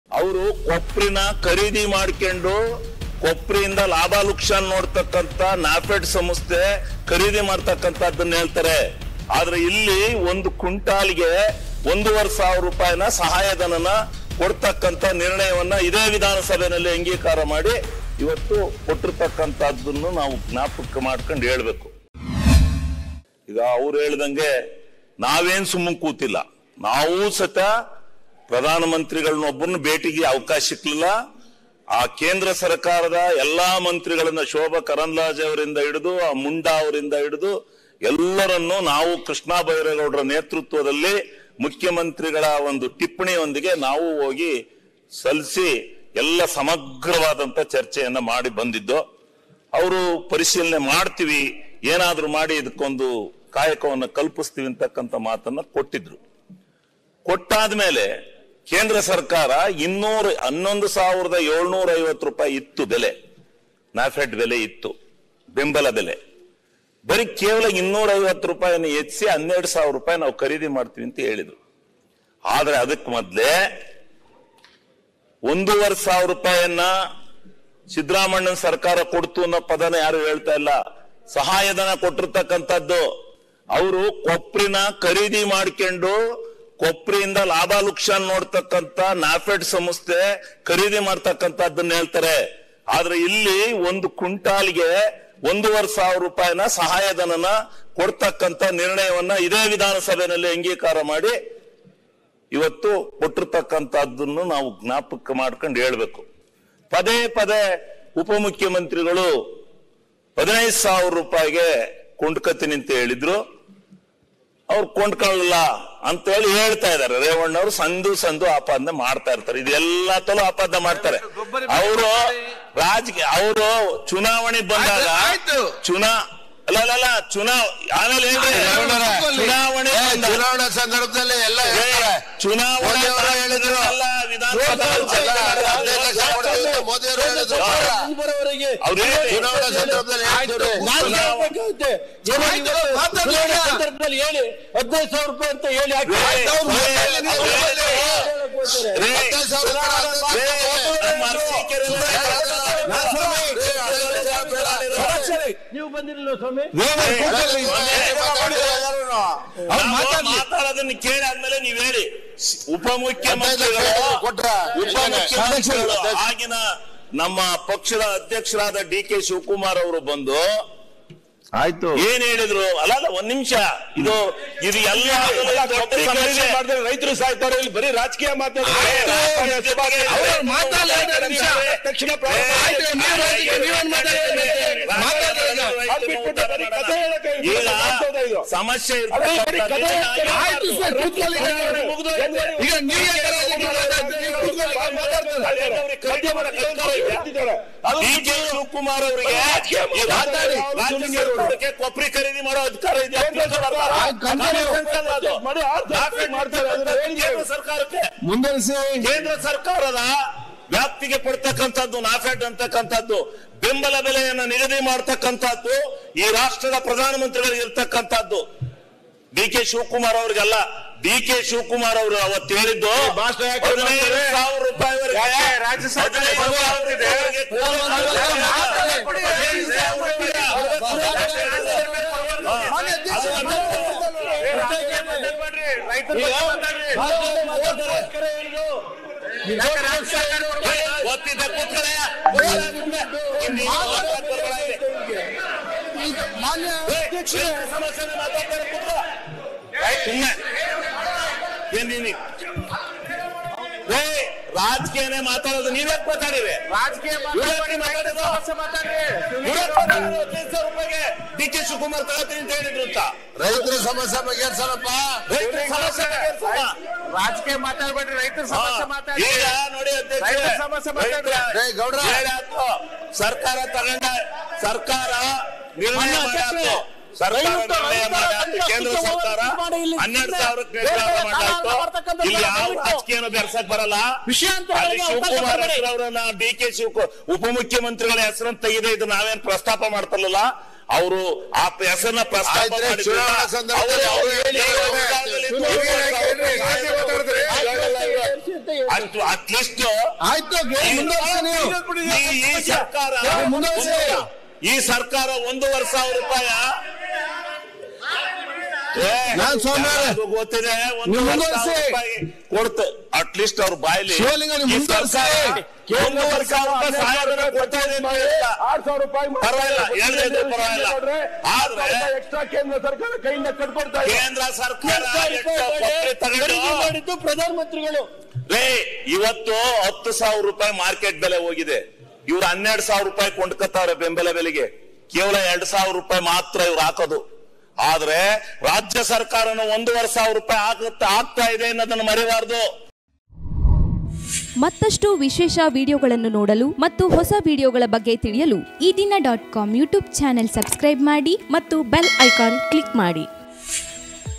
Assembly Session | ಕೊಬ್ಬರಿಗೆ ಬೆಂಬಲ ಬೆಲೆ; ಶಿವಲಿಂಗೇಗೌಡ & ಸುರೇಶ್ ಗೌಡ ನಡುವೆ ಜಟಾಪಟಿ | Shivalingegowda
ವಿಧಾನ ಸಭೆ ಅಧಿವೇಶನದ ಇಂದಿನ ಕಾರ್ಯಕಲಾಪದಲ್ಲಿ ಕೊಬ್ಬರಿಗೆ ಬೆಂಬಲ ಬೆಲೆ ನೀಡುವ ವಿಷಯಕ್ಕೆ ಸಂಬಂಧಿಸಿದಂತೆ  ಶಾಸಕ ಕೆಎಂ ಶಿವಲಿಂಗೇಗೌಡ  ಮತ್ತು ಶಾಸಕ ಸುರೇಶ್ ಗೌಡ ನಡುವೆ ಬಿರುಸಿನ ವಾಗ್ವಾದ ನಡೆಯಿತು.